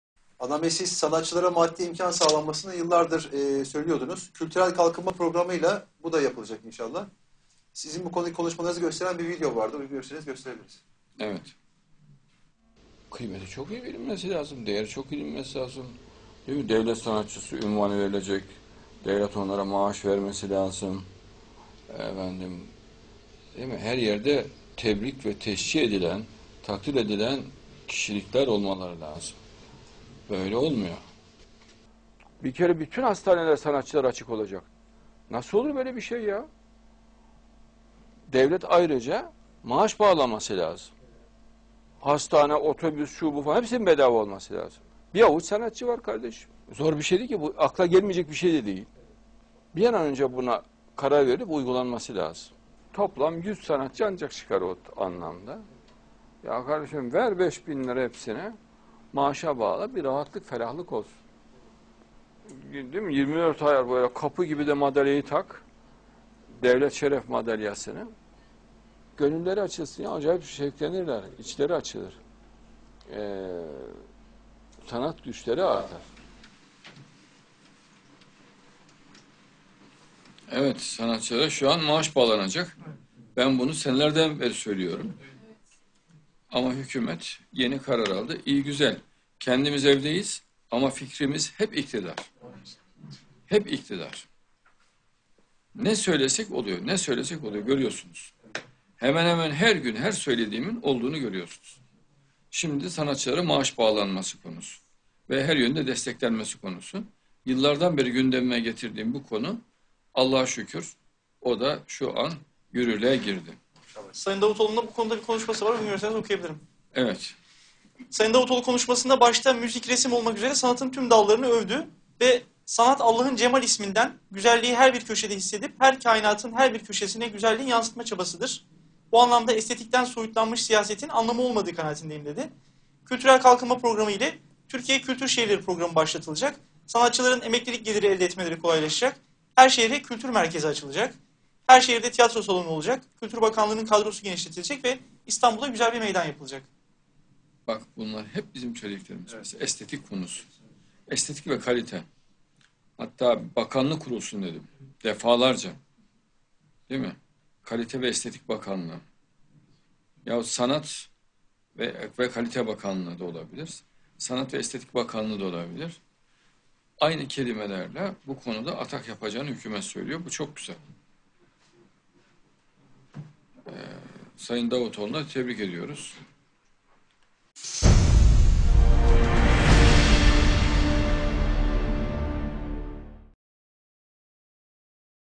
Sn. Adnan Oktar’ın yıllardır söylediği sanatçılara maddi imkan sağlanması konusu gerçekleşiyor
Adnan Oktar’ın 22 Nisan 2016 tarihli A9 TV röportajından